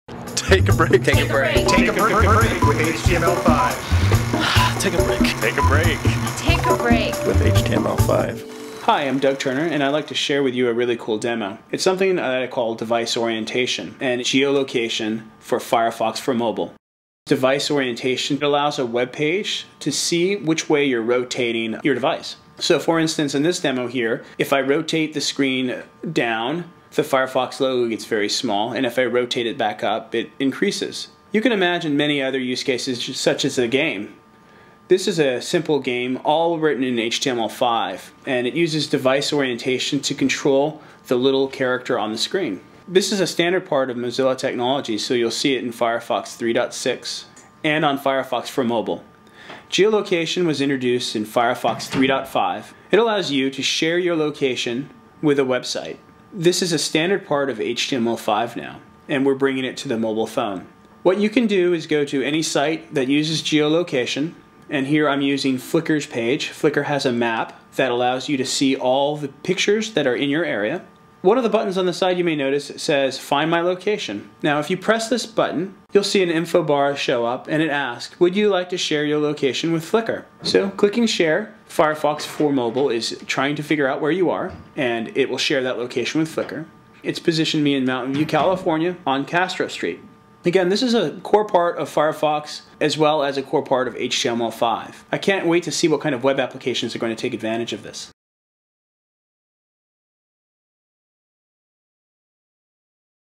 HTML5 in Firefox: Device Orientation and Geolocation on the Nokia N900
Doug Turner demos device device orientation and geolocation on the Nokia N900.